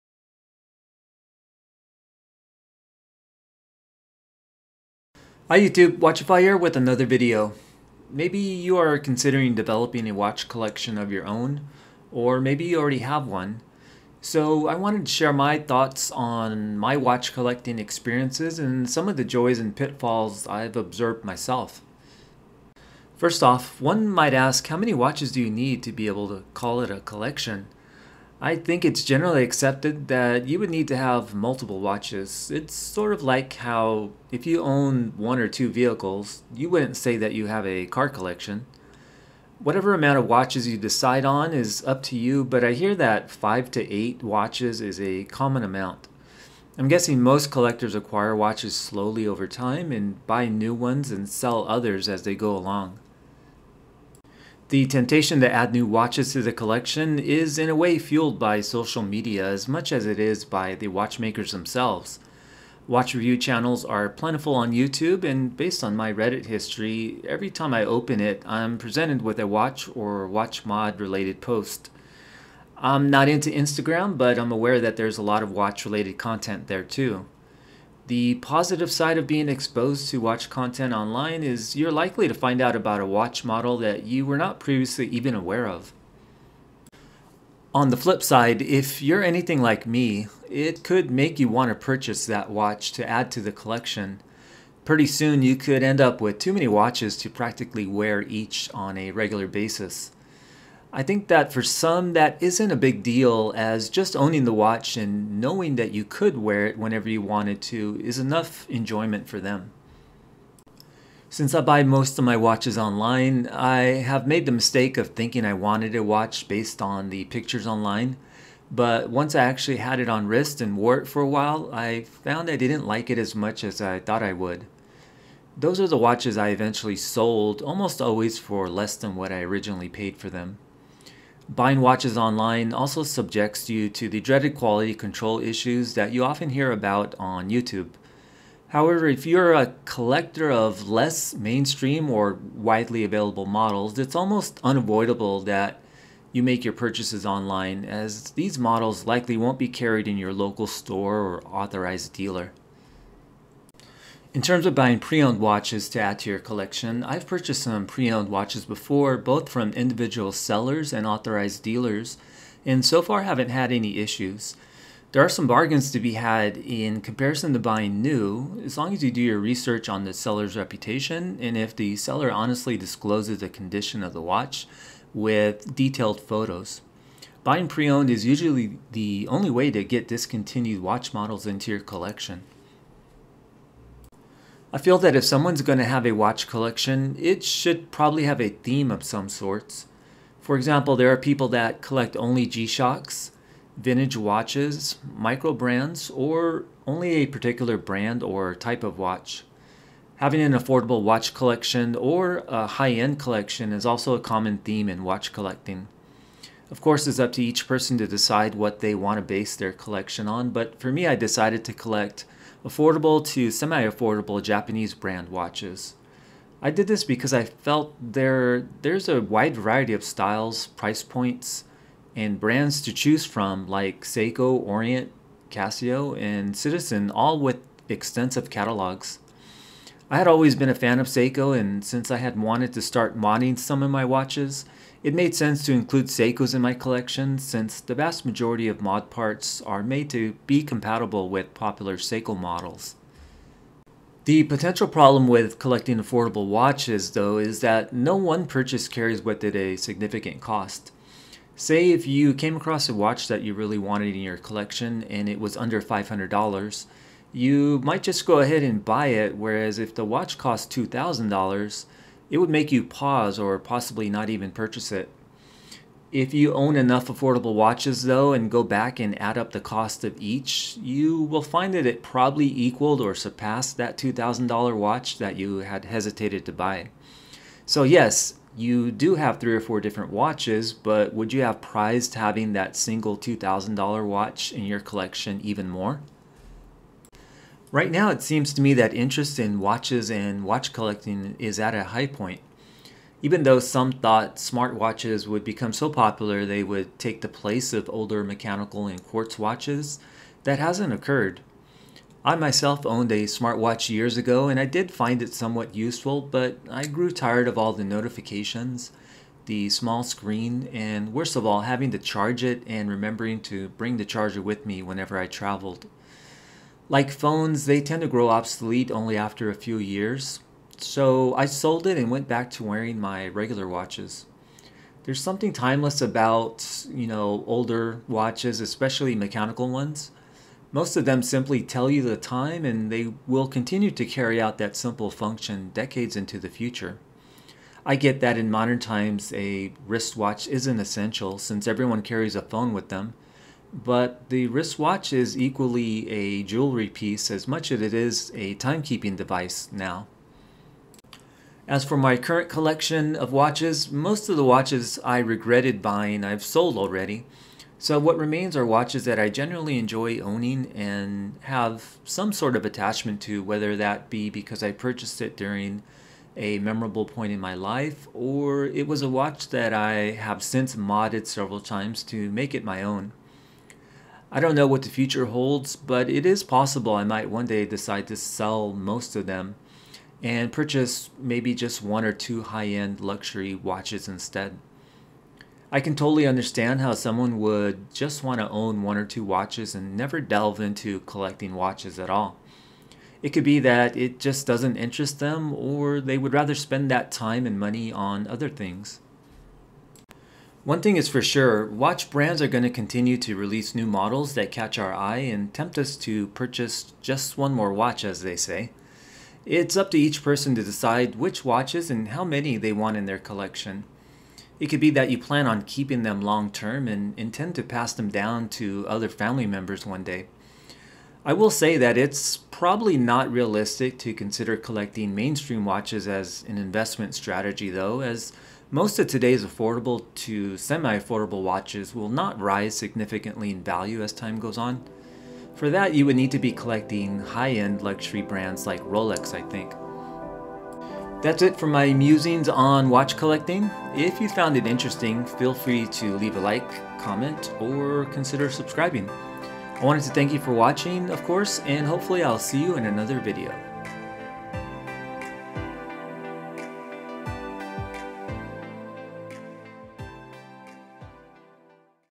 My Watch Collecting Thoughts
Watch collecting seems as popular as ever.  I thought I would post a short video sharing my thoughts and experiences on the hobby of watch collecting